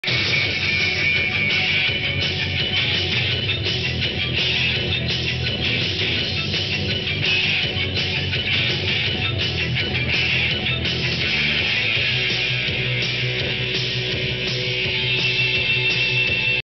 Romain 60 YD RunDon Bosco vs.Paul VI States
Dillon Romain 2nd round of states PaulVI